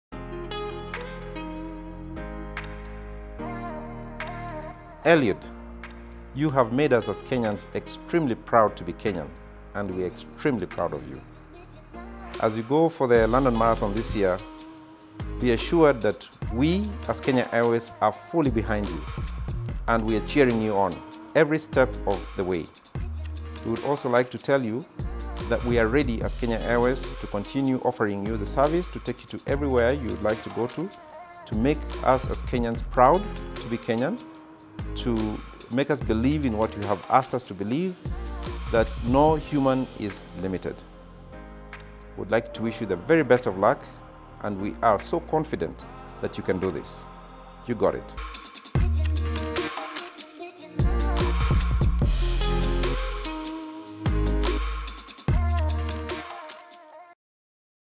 All the best Eliud at LondonMarathon2020
KENYA STANDS BEHIND YOU ELIUD
All the best as you work toward flying our flag high. We love you and cheer you on.

Go Eliud! Go!